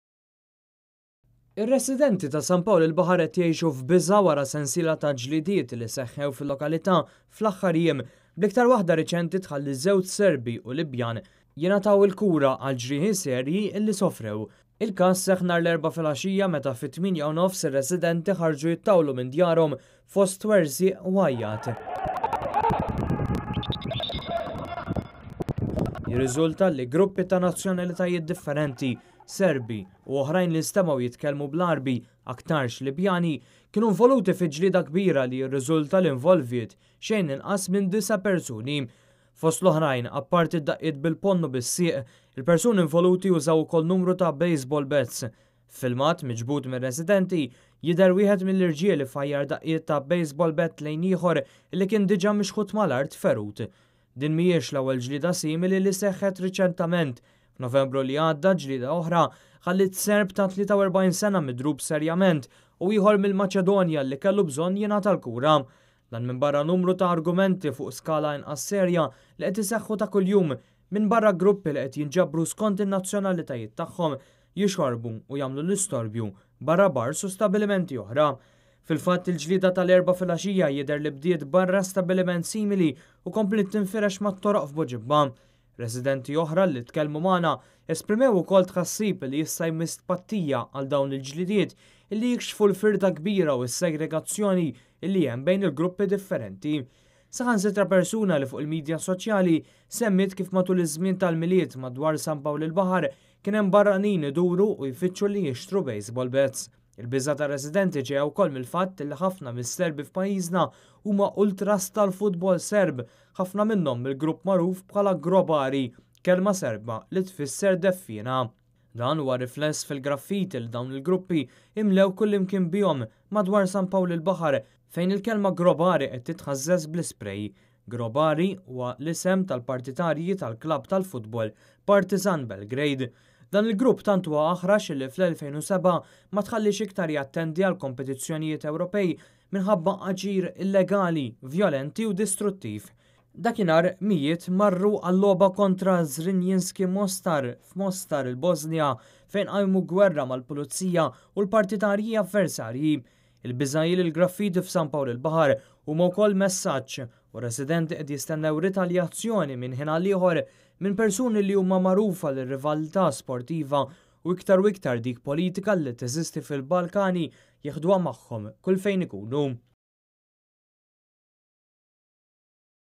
ĠLIEDA F'SAN PAWL IL-BAĦAR - QED JISTENNEW GWERRA
Residenti ta’ San Pawl il-Bahar huma bezghana minn dik li qed isejhu bhala gwerra li tista’ tinqala’ bejn gruppi ta’ nazzjonalitajiet differenti fiz-zona.

Il-glieda li sehhet l-Erbgha kienet biss l-iktar wahda ricenti minn sensiela.